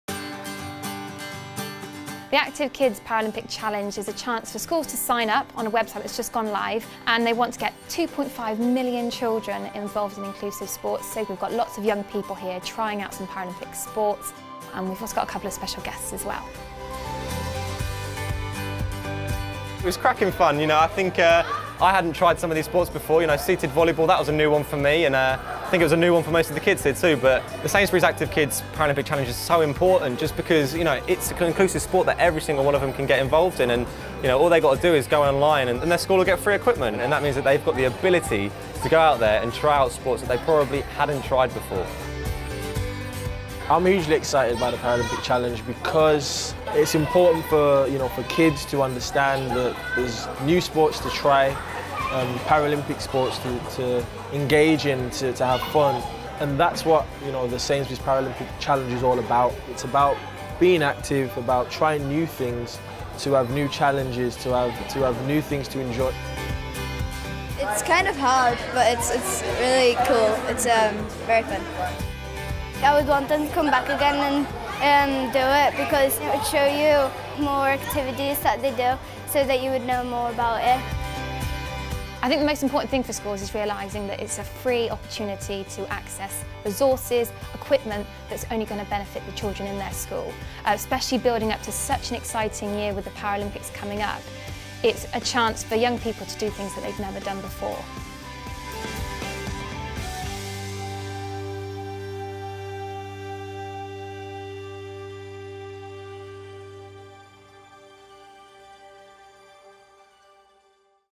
Daniel Sturridge and Jonnie Peacock launch Sainsbury’s Active Kids Paralympic Challenge
Daniel Sturridge joined Paralympic champion Jonnie Peacock to launch a new initiative to get more young people involved in inclusive sport and to raise awareness of Paralympic sport ahead of the Rio 2016 Games.

Sainsbury’s Active Kids Paralympic Challenge has been designed to introduce 2.5 million young people to the sports of boccia, goalball, sitting volleyball and athletics, offering schools the opportunity to win money can’t buy prizes, including a trip to the Rio 2016 Paralympic Games. To find out more about how your school could benefit from free equipment and online resources